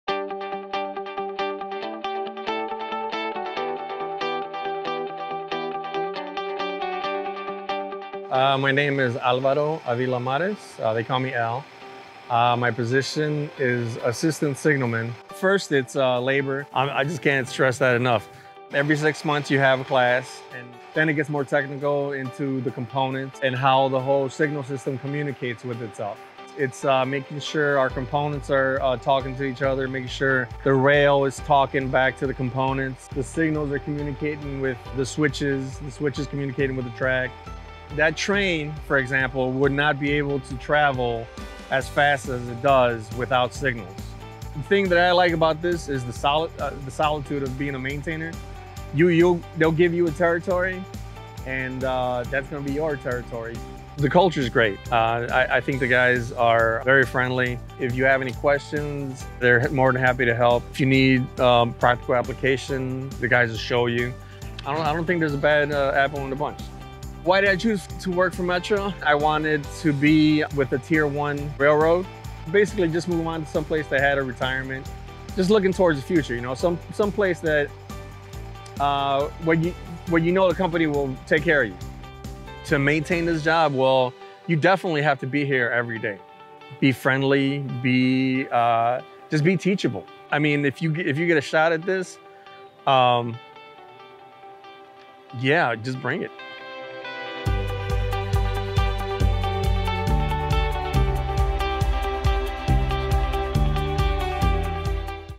Assistant Signalman | Metra Career Highlight
Assistant Signalman Alvaro Mares shares some insights into what it is like to work in his field and have a career at Metra.